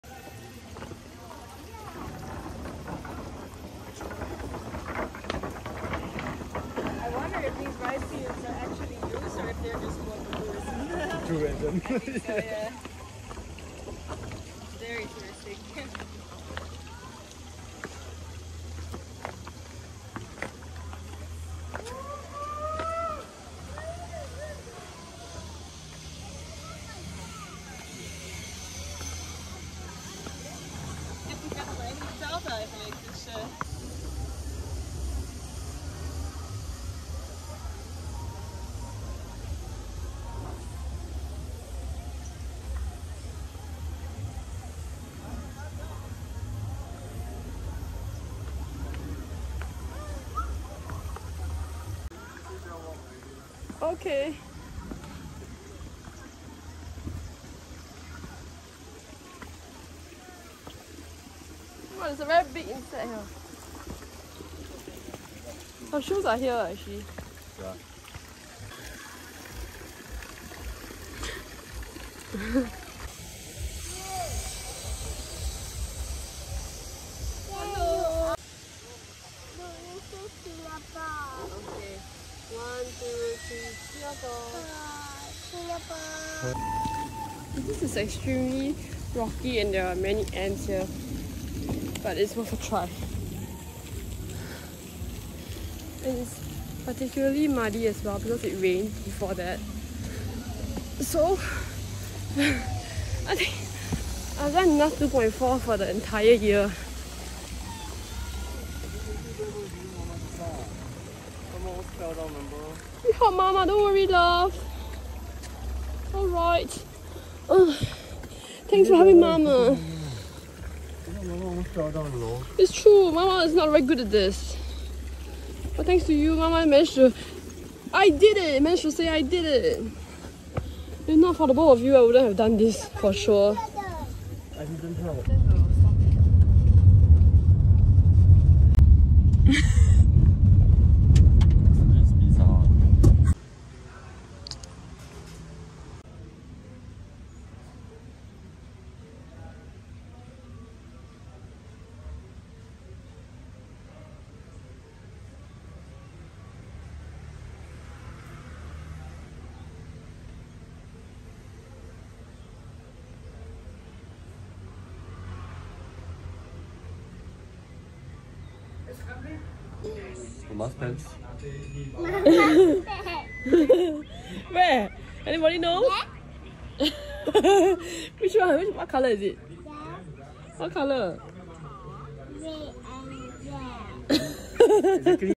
Exploring Tegalalang Rice Terrace In Bali
UNESCO World Heritage site, comprising of a series of beautiful cascading rice fields.

Tegalalang Rice Terrace
Location: Jl. Raya Tegallalang, Tegallalang, Kec. Tegallalang, Kabupaten Gianyar, Bali 80561, Indonesia

Family -   / rachelandvera

Family -   / rachelandvera